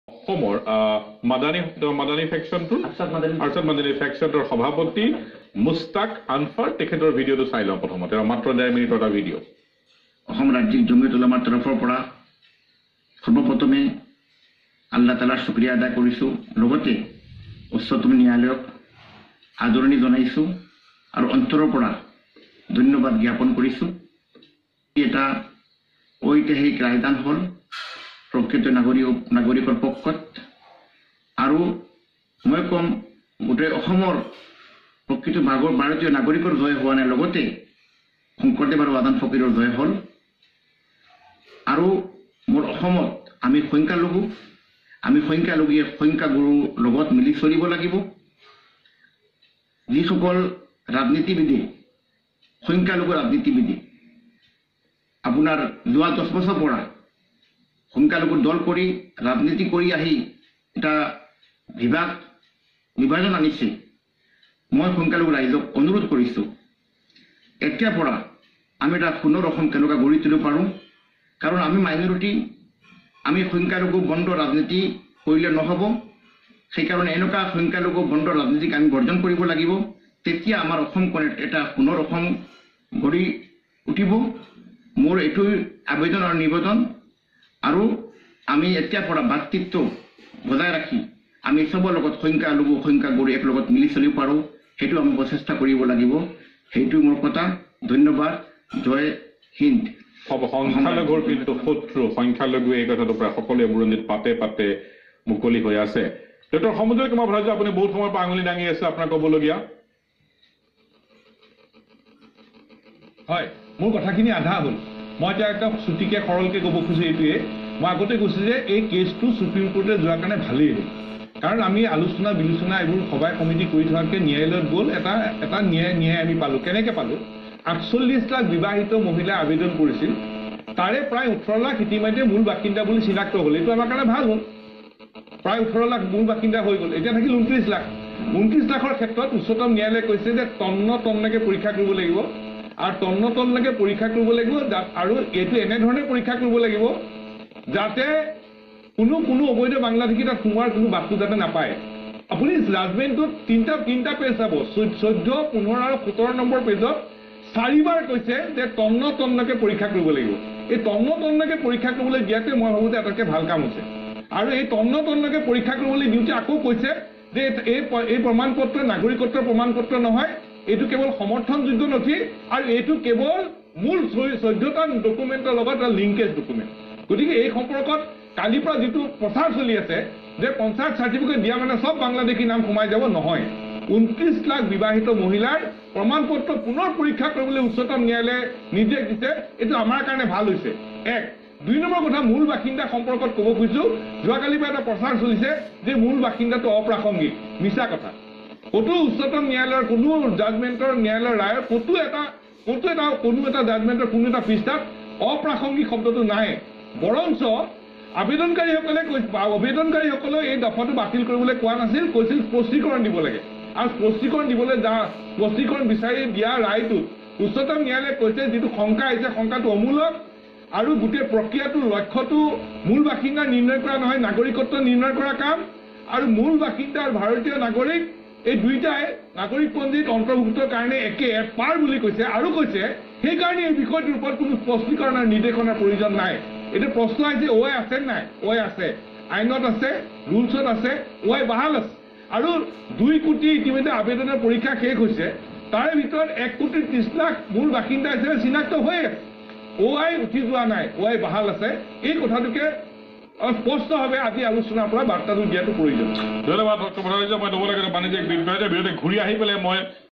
Maulana Mushtaque Anfar on Pratidin Time Assam News
Live debate program on Pratidin Time News 06/12/17